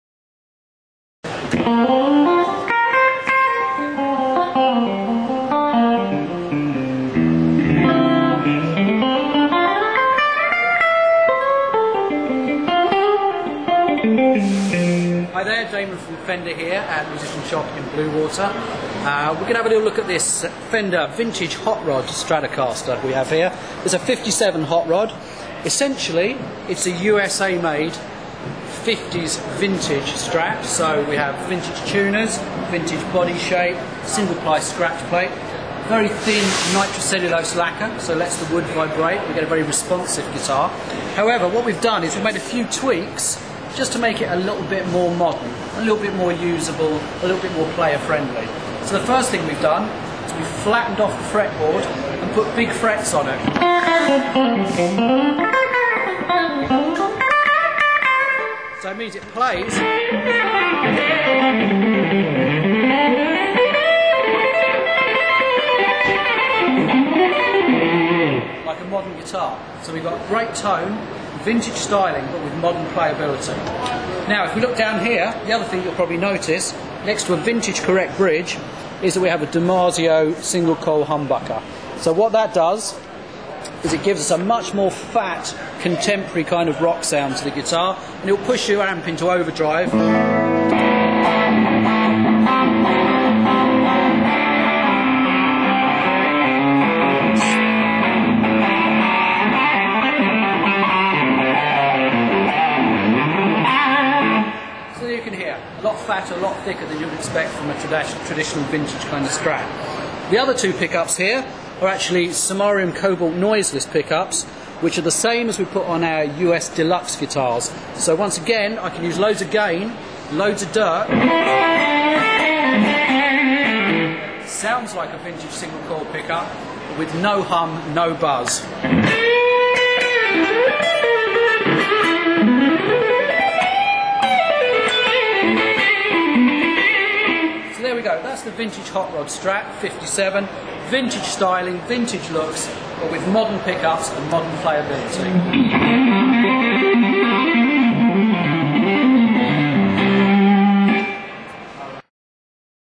Fender 57 Hot Rod Strat from MusicianShop.com
Fender's product specialist Damon, playing a Fender 57 Hot Rod Sratocaster.